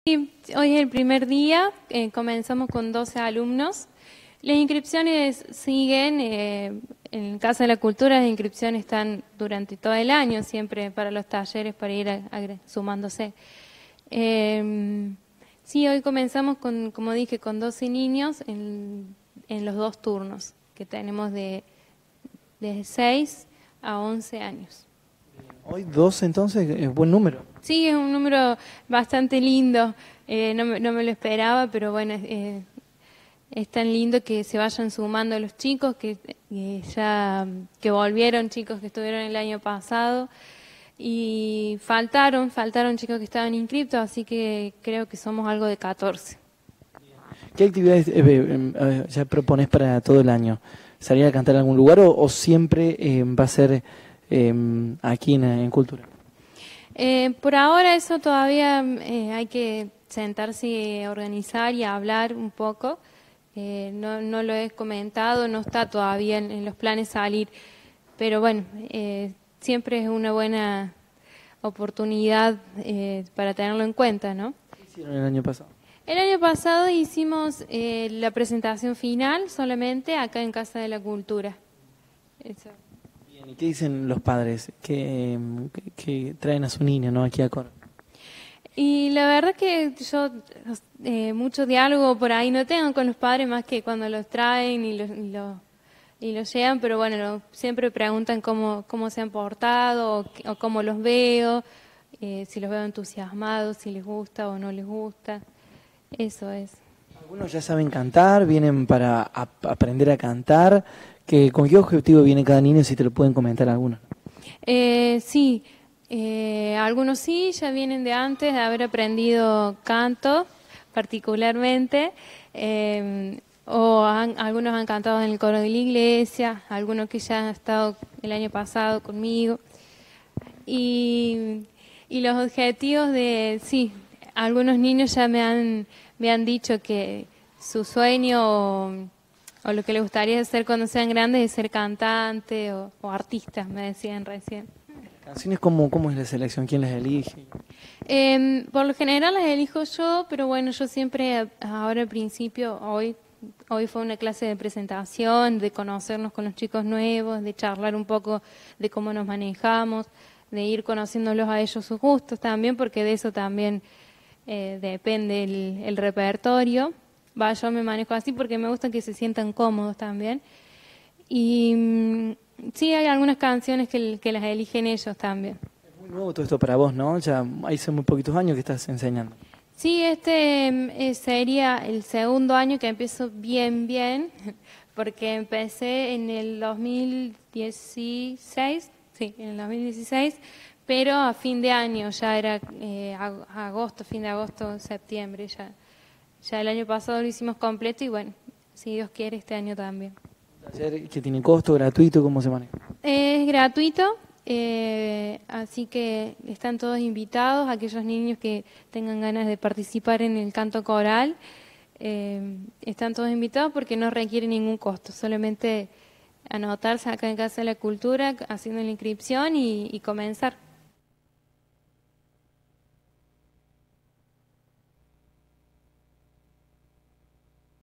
COMENZARON LAS CLASES DE CORO PARA NIÑOS EN CULTURA
GENERAL DEHEZA: COMENZARON LAS CLASES DE CORO PARA NIÑOS EN CULTURA